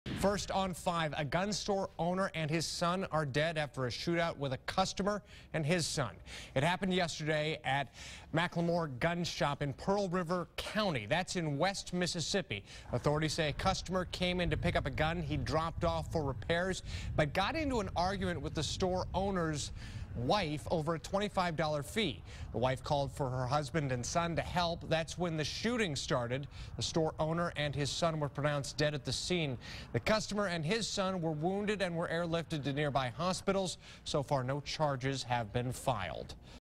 Two Dead After Shooting at Mississippi Gun Store
The shooting may have stemmed from a disagreement over a fee.